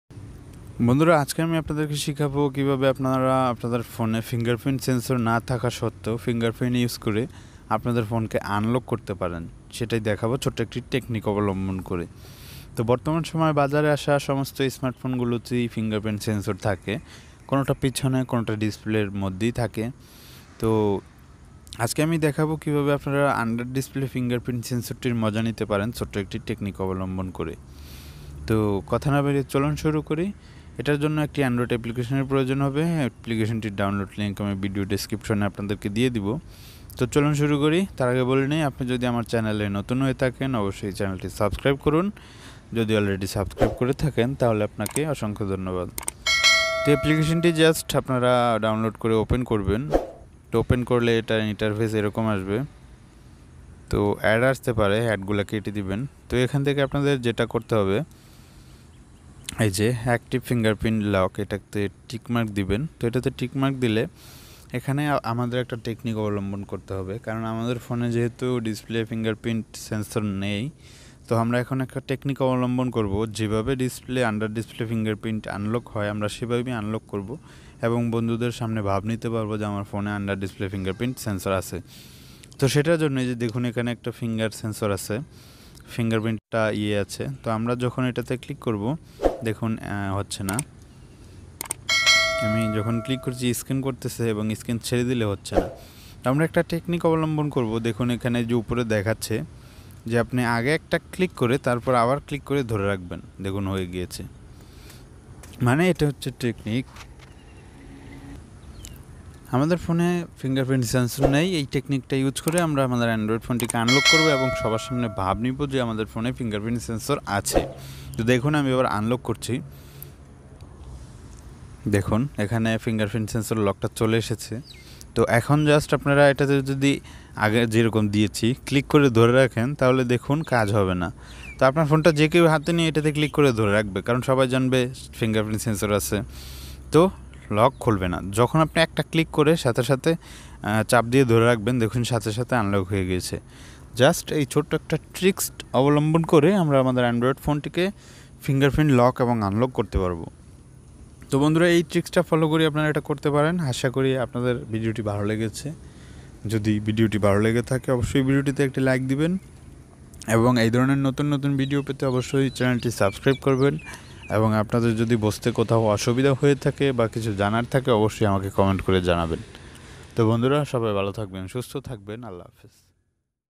ফিঙ্গারপ্রিন্ট লক ব্যবহার করুন যে কোন এন্ড্রয়েড ফোনে
আজকে আপনাদেরকে দেখাব কিভাবে ফিঙ্গারপ্রিন্ট সেন্সর না থাকা সত্বেও আপনার ফোনে ফিঙ্গারপ্রিন্ট আনলক এর মজা নিতে পারেন ছোট্ট একটি টেকনিক অবলম্বন করে তো সবটা বিষয়ে জানার জন্য এবং শিখার জন্য অবশ্যই পুরো ভিডিওটি দেখতে থাকুন এবং এই ধরনের নতুন ভিডিও পেতে আমাদের চ্যানেলটি সাবস্ক্রাইব করে রাখুন